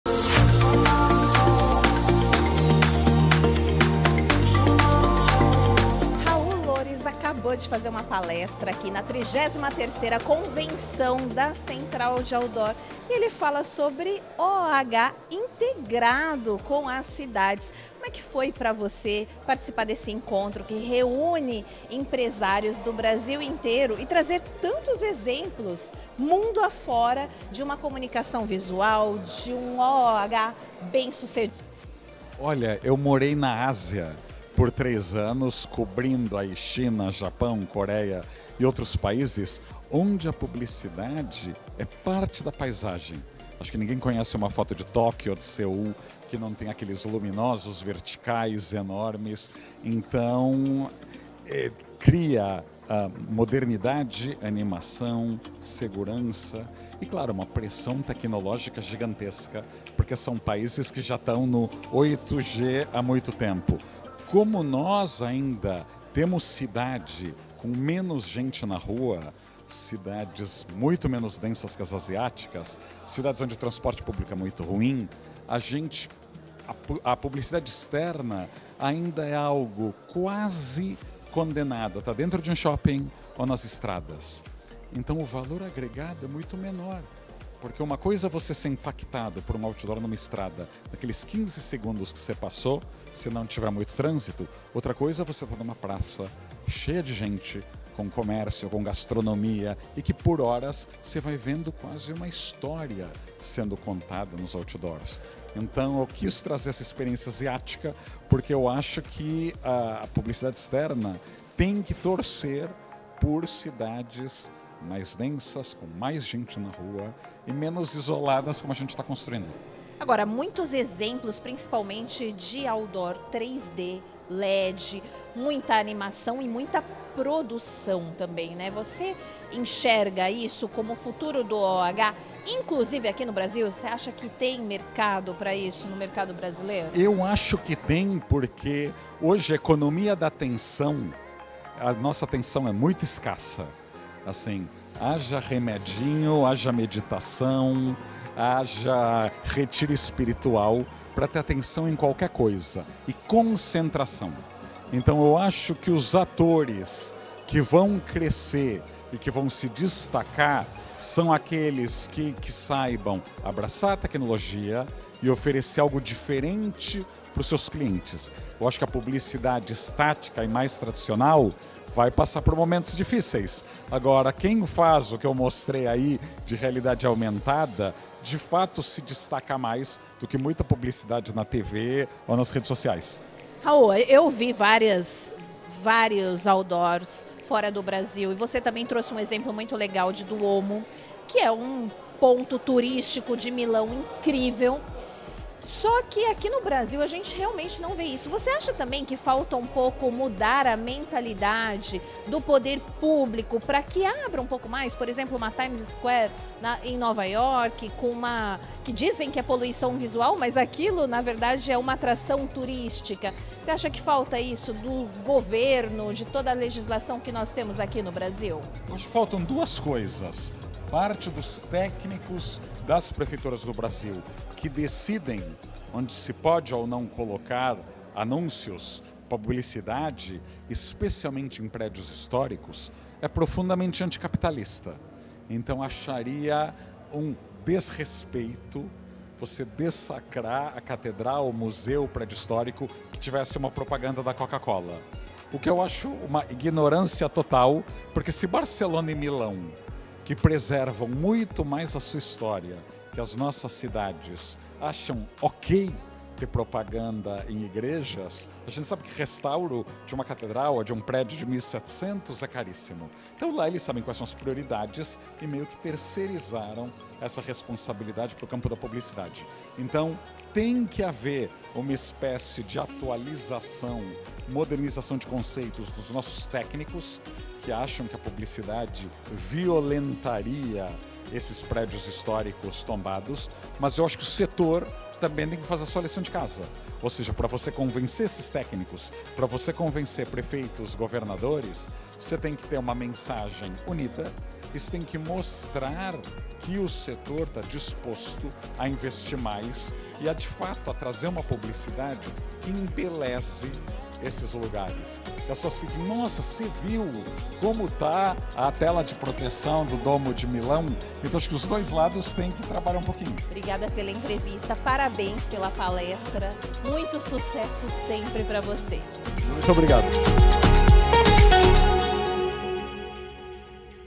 Raul Lores destaca integração do OOH com as cidades na Convenção da Central de Outdoor
Raul Juste Lores, jornalista, autor do projeto São Paulo nas Alturas, participou como conferencista da 33ª Convenção da Central de Outdoor, realizada em Florianópolis-SC. Em um encontro que reuniu empresários de todo o Brasil, Lores compartilhou insights valiosos na palestra "Integração da Mídia OOH nas Cidades do Futuro".

Com uma experiência de três anos cobrindo países como China, Japão, Coreia e outras nações onde a publicidade é uma parte intrínseca da paisagem urbana, Lores trouxe exemplos inspiradores de como a comunicação visual bem-sucedida pode transformar as cidades.

Em sua palestra, Raul destacou as diferenças entre as abordagens asiáticas e brasileiras em relação à mídia exterior. Enquanto em países como China e Japão a publicidade externa é uma presença marcante em grandes pontos turísticos, no Brasil ela ainda é limitada e resulta em um valor agregado menor. "A publicidade externa tem que torcer por cidades mais densas, com mais gente na rua e menos isoladas, como estamos construindo", ressalta.

Quando questionado sobre o futuro do OOH no Brasil, Lores expressou otimismo, destacando a crescente escassez de atenção das pessoas na economia moderna.  "Eu acho que os atores que vão crescer e se destacar são aqueles que saibam abraçar a tecnologia e oferecer algo diferente para os seus clientes. A publicidade estática e mais tradicional vai passar por momentos difíceis.

Um dos exemplos que Raul Lores apresentou essa palestra foi uma publicidade na Catedral de Duomo, um dos principais pontos turísticos de Milão, na Itália, onde parte do valor do restauro é proveniente da publicidade.

Além disso, em entrevista com a jornalista Elisangela Peres, Raul Lores comentou sobre a legislação e mentalidade do poder público e de empresários em relação à mídia exterior. "Faltam duas coisas: parte dos técnicos das prefeituras do Brasil que decidem onde se pode ou não colocar anúncios, publicidade, especialmente em prédios históricos, é profundamente anticapitalista, e acharia um desrespeito você dessacrar a Catedral, museu e prédio histórico que tivesse uma propaganda da Coca-Cola, por exemplo. O que eu acho uma ignorância total. Sabemos que restauro de uma catedral, de um prédio de 1700, é caríssimo. Então, Milão e Barcelona sabem quais são as prioridades e meio que terceirizaram essa responsabilidade para o campo da publicidade. Tem que haver uma espécie de atualização, modernização de conceitos dos nossos técnicos que acham que a publicidade violentaria esses prédios históricos tombados. Mas, eu acho que o setor também tem que fazer a sua lição de casa, ou seja, para você convencer esses técnicos, prefeitos, governadores, você tem que ter uma mensagem unida e tem que mostrar que o setor está disposto a investir mais e, de fato, trazer uma publicidade que embeleze esses lugares".